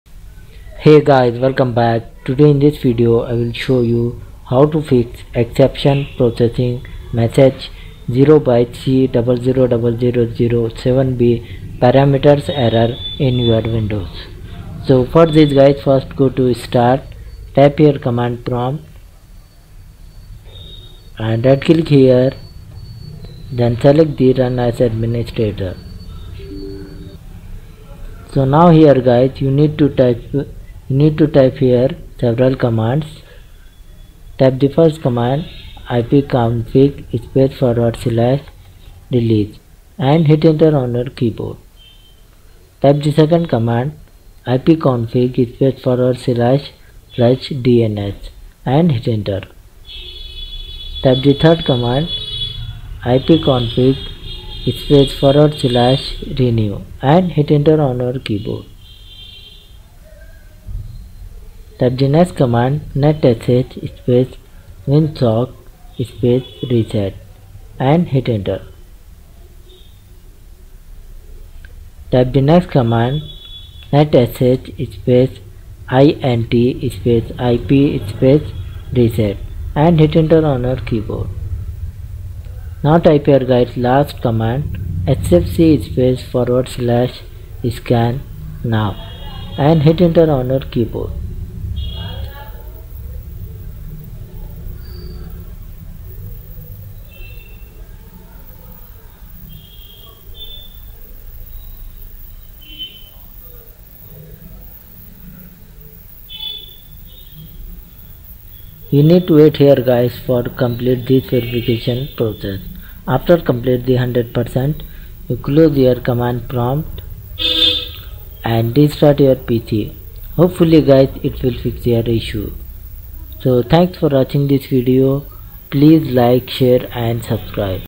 Fix Exception Processing Message 0xc000007b Parameters Error
In this Tutorial I Will show you, Fix Exception Processing Message 0xc000007b Parameters Error.